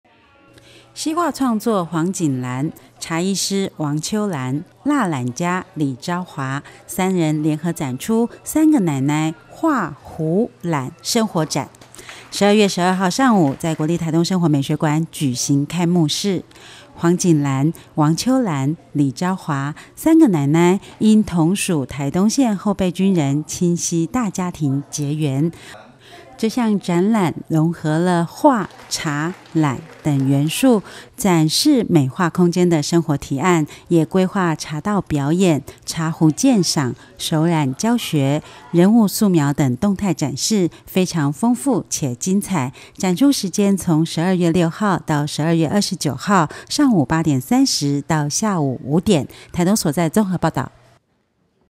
2014 12 12 「三個奶奶 畫壺染生活展」 利貞傳播-台東所在-台東新聞
西畫創作黃錦蘭、茶藝師王秋蘭、蠟染家李招華
三人聯合展出「三個奶奶-畫壺染生活展」
12月12日上午，在國立臺東生活美學館舉行開幕式

黃錦蘭、王秋蘭、李招華
「三個奶奶」因同屬臺東縣後備軍人青溪大家庭結緣
這項展覽融合了畫、茶、染等元素
展示美化空間的生活提案
也規劃茶道表演、茶壺鑑賞、手染教學、
人物素描等動態展示，非常豐富且精彩
展出時間從12月6日至12月29日，上午8點30至下午5點